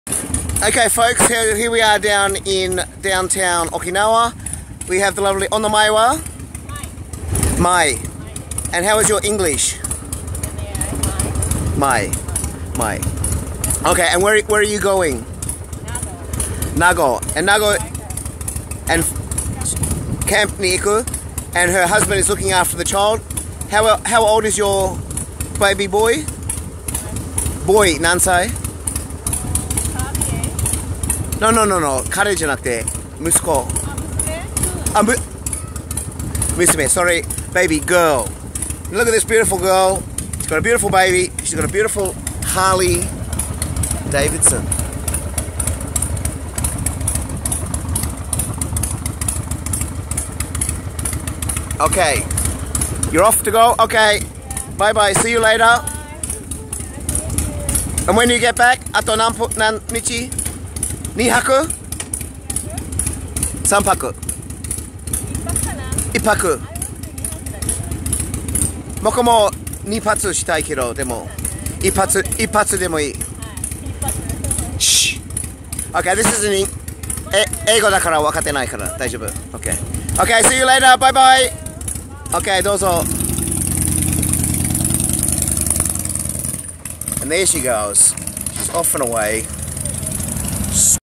RWC 2019 Break in Okinawa. Part 2
Here I interview the Japanese man, ATM's, wife at American Village before she heads out on her Harley Davidson for a night camping.